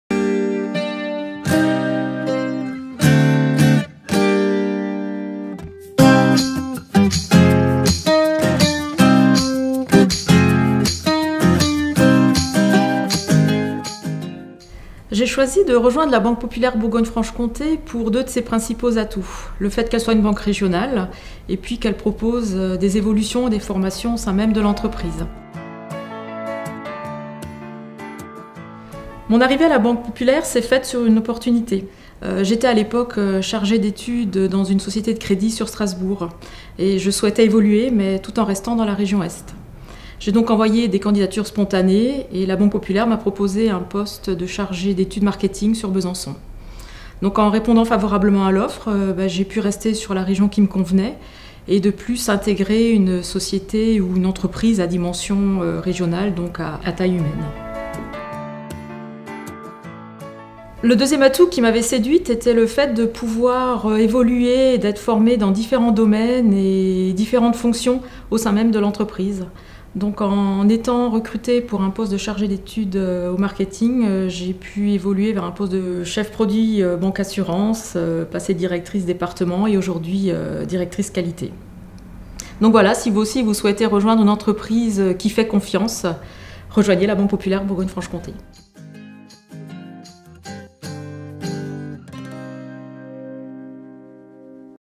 Une banque coopérative et mutualiste - Annik STUDER - BPBFC Talents
Découvrez sur notre site de recrutement de nombreuses offres d'emploi et parcourez les fiches métiers de la Banque Populaire Bourgogne Franche-Comté.
Aujourd'hui découvrez Annik STUDER, Directrice Qualité, à la Banque Populaire Bourgogne Franche-Comté.